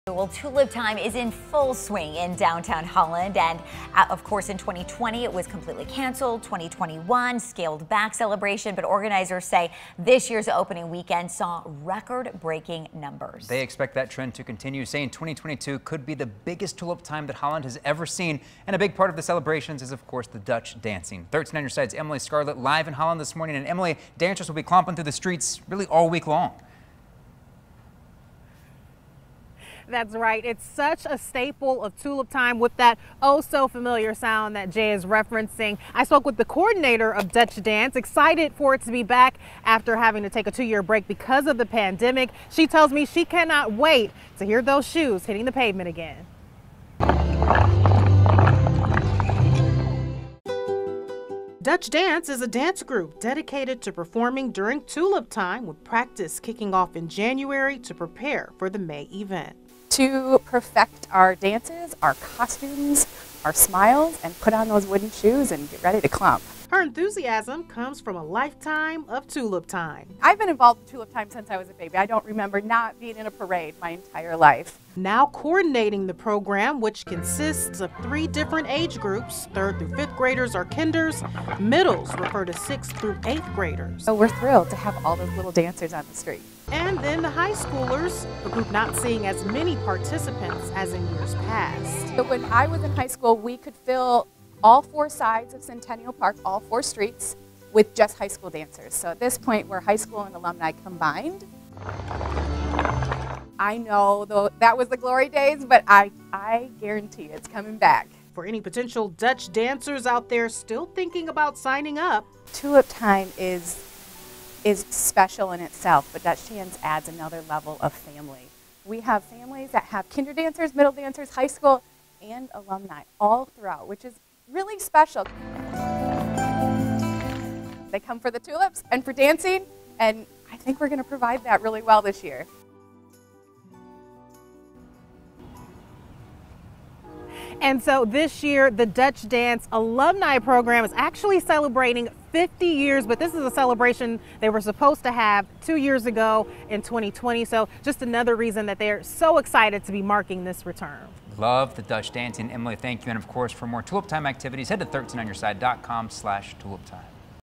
A closer look at Dutch Dances during Tulip Time
Dutch Dance is a group dance dedicated to performing during Tulip Time, with practice kicking off in January to prepare for the May event.

We took a closer look at the iconic dances.